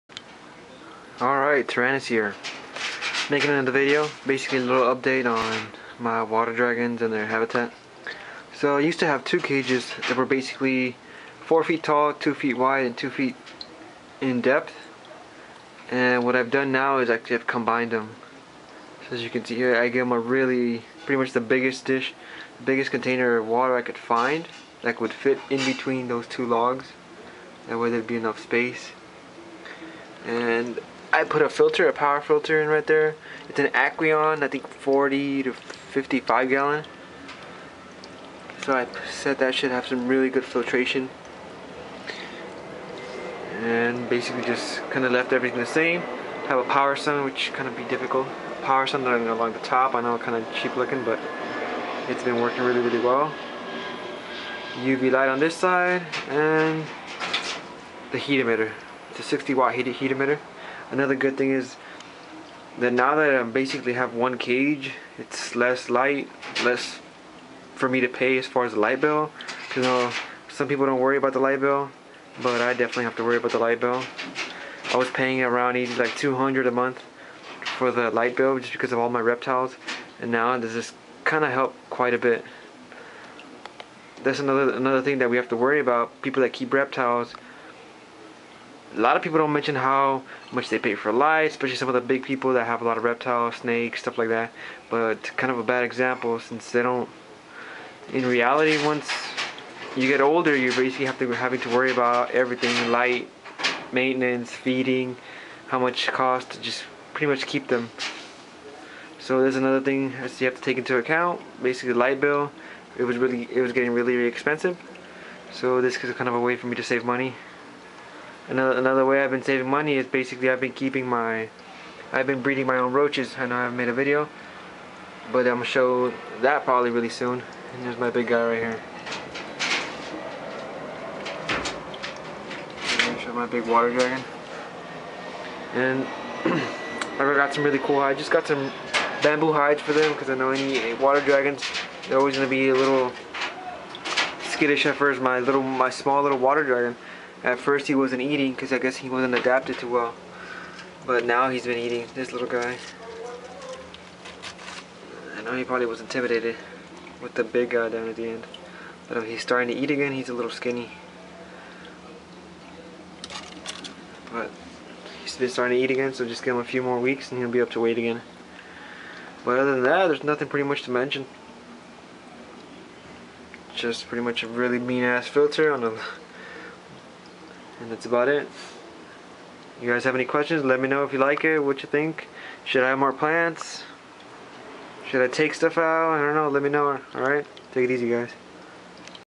How to set up a Chinese Water Dragon Set Up (Update)
Combined two of my cages into one to help reduce costs